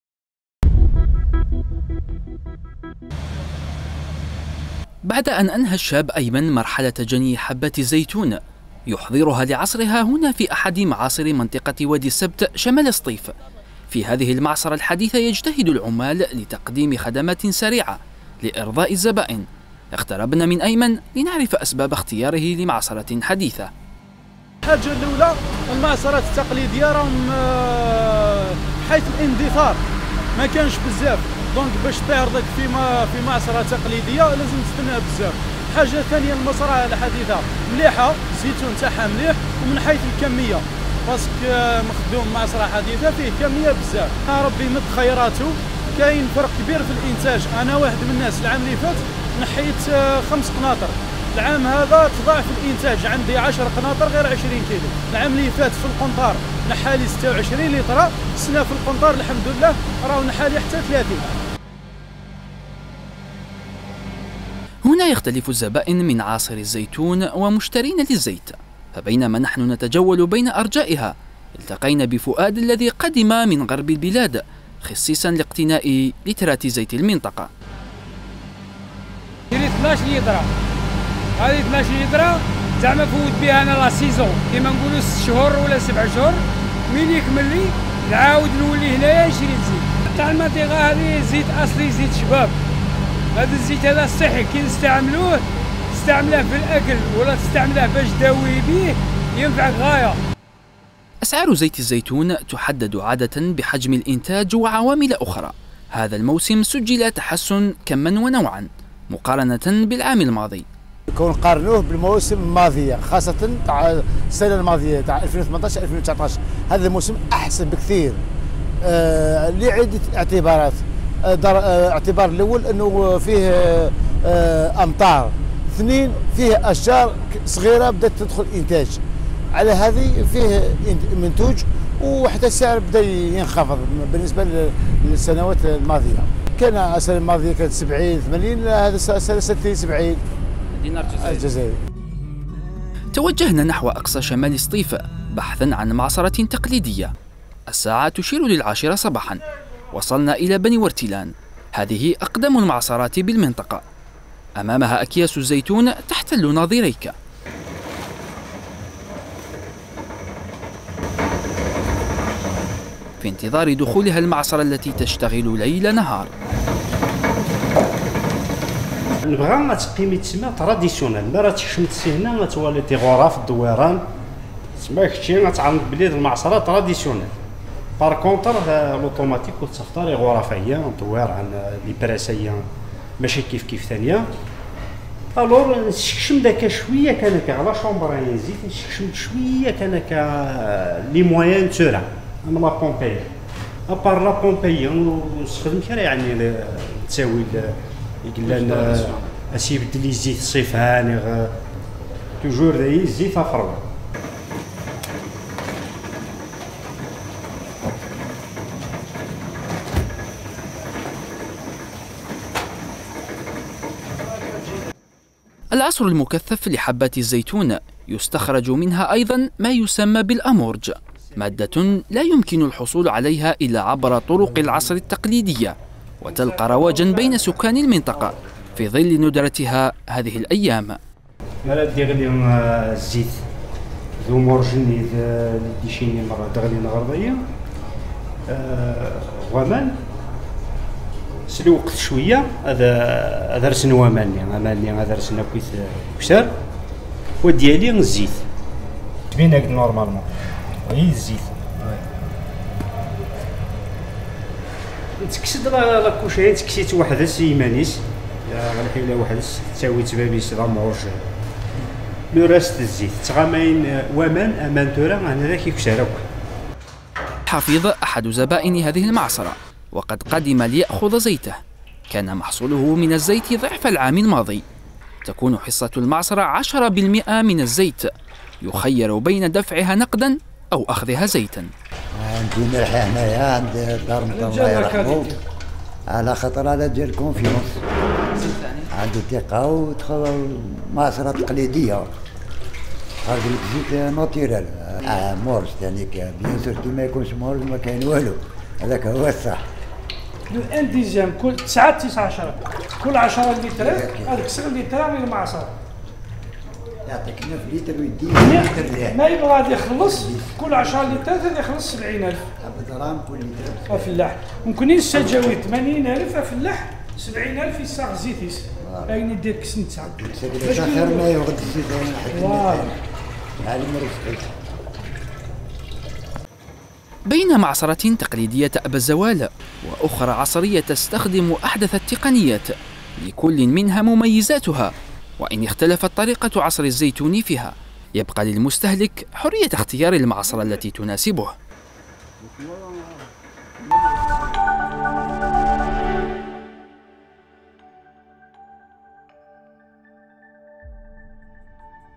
زيت الزيتون في سطيف.. بين معاصر تقليدية وحديثة، أيهما أجود !
سطيف (وأج)- تتنوع معاصر الزيتون بين تقليدية وحديثة في شمال سطيف، لكل منهما خصائص ويبقى الاختيار للمستهلك.